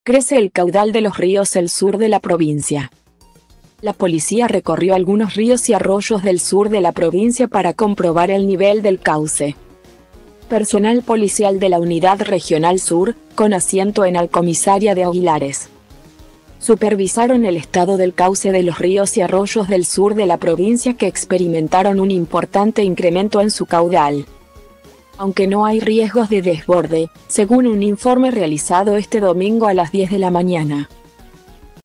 Crece el caudal de los ríos el sur de la provincia
La Policía recorrió algunos ríos y arroyos del sur de la provincia para comprobar el nivel del cauce. Personal policial de la Unidad Regional Sur, con asiento en al comisaria de Aguilares,supervisaron el estado del cauce de los ríos y arroyos del sur de la provincia que experimentaron un importante incremento en su caudal,